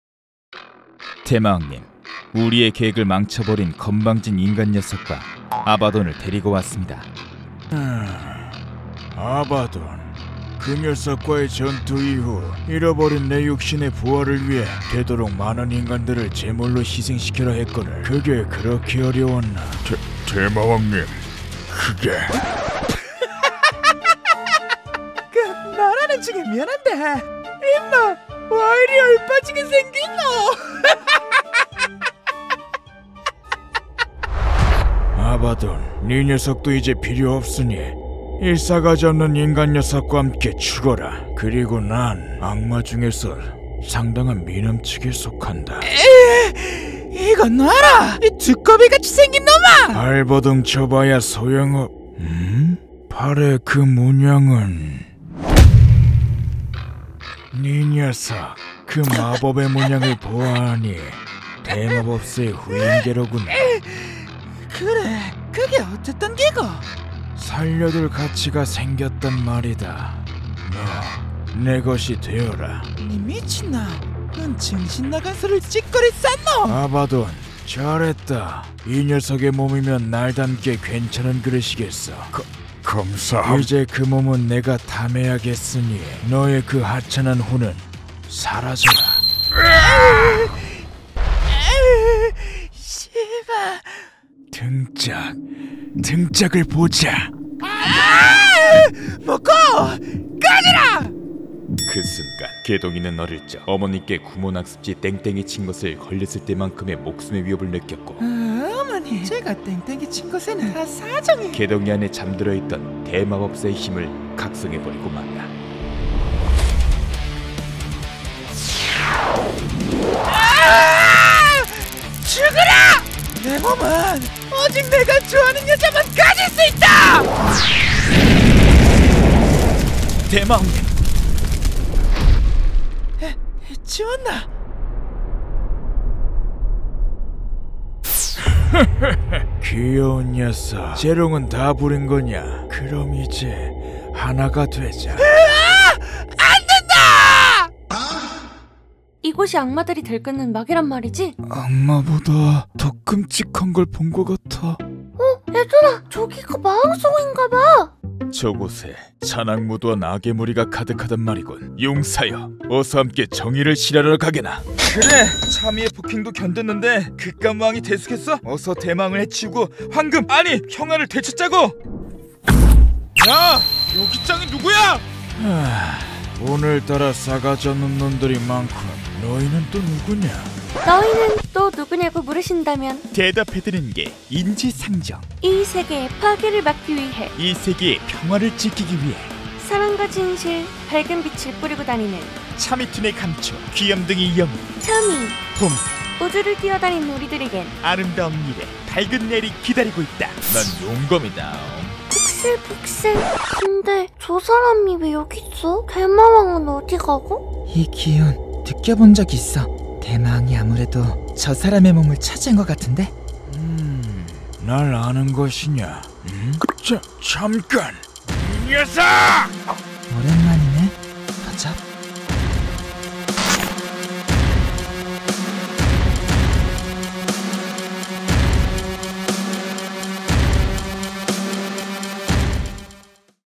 [차미툰 외전] 대마왕
[차미툰 외전] - 이세계에서의 만남 시리즈
평범한 고등학생이 이세계에서 용사가 된다면..?!

💜해당 영상에 사용된 서체는 '헤움 디자인'의 폰트입니다.💜